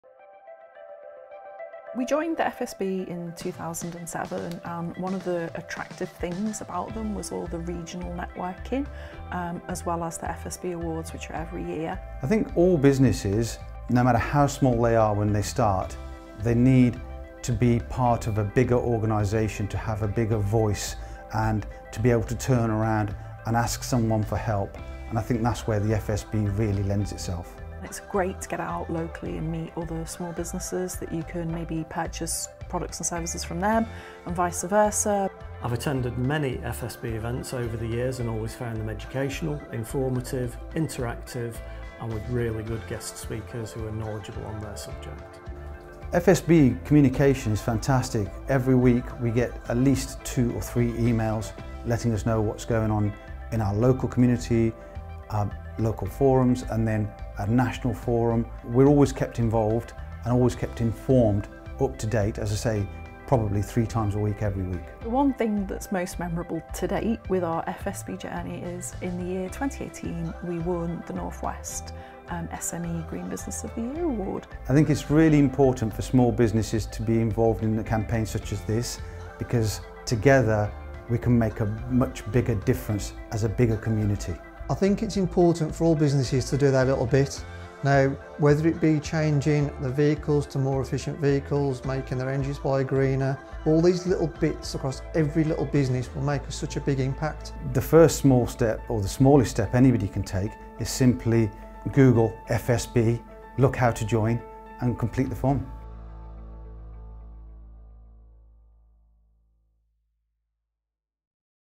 How we're supporting Businesses Across the UK | FSB Membership
Hear from small business owners Becky Toal, Graham Oakley, Lash Saranna and Stephen Dootson about their experience of FSB membership, the value it brings their business and their most memorable moments of membership.

Connect with fellow business owners at regular networking events and stay up to date with what’s happening locally and nationally in the small business world. Discover just some of the many benefits of FSB membership.